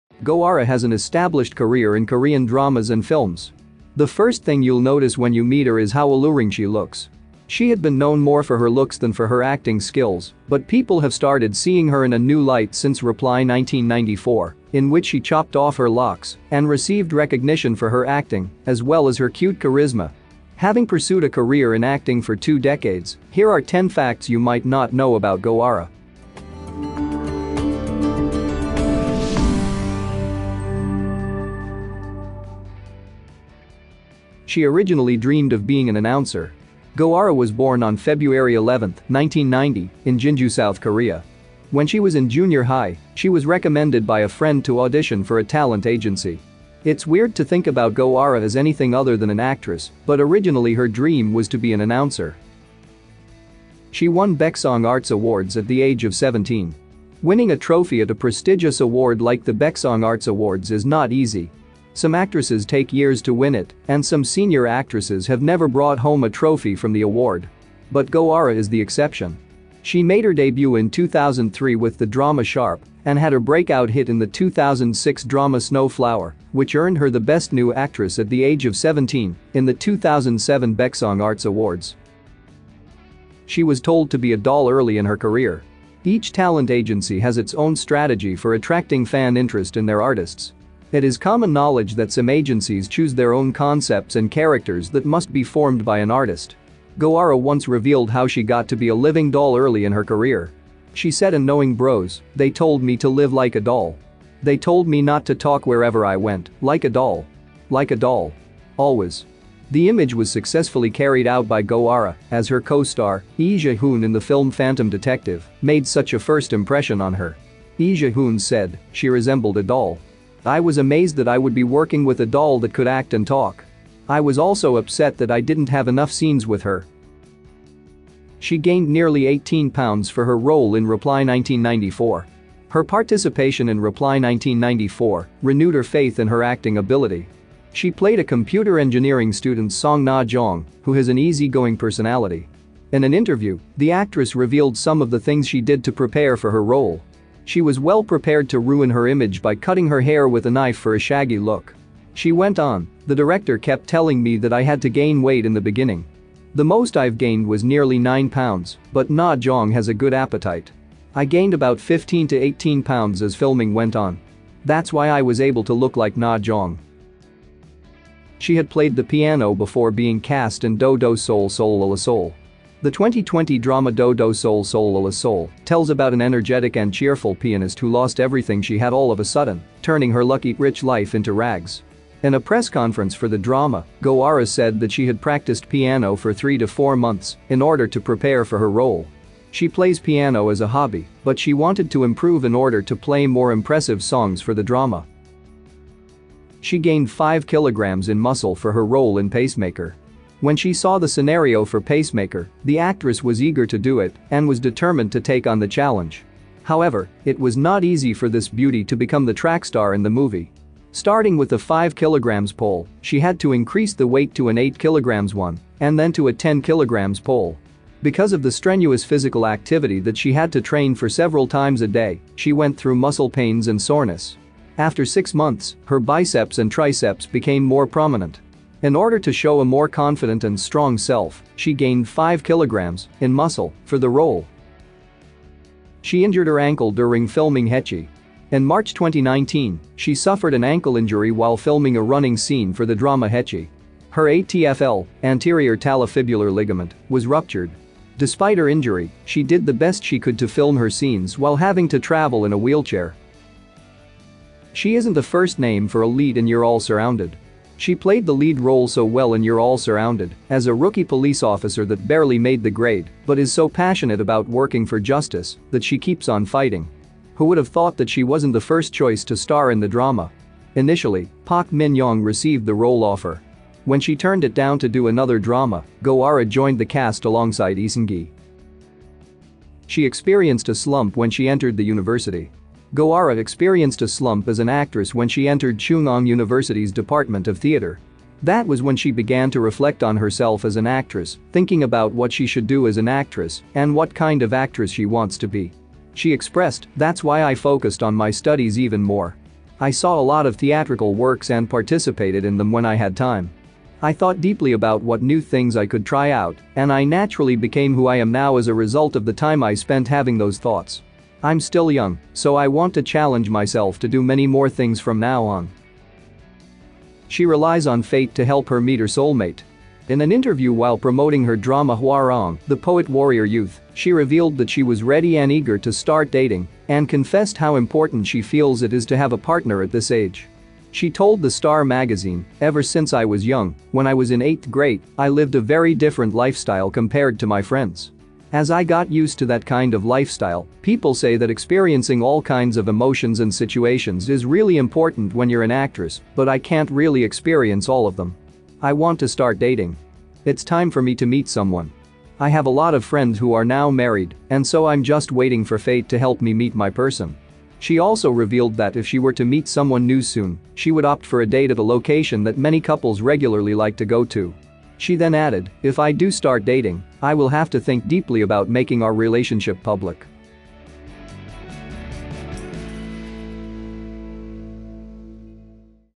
10 Things You Didn't Know About Go Ara (고아라) | Star Fun Facts
Some things you probably didn't know about Go Ara (고아라)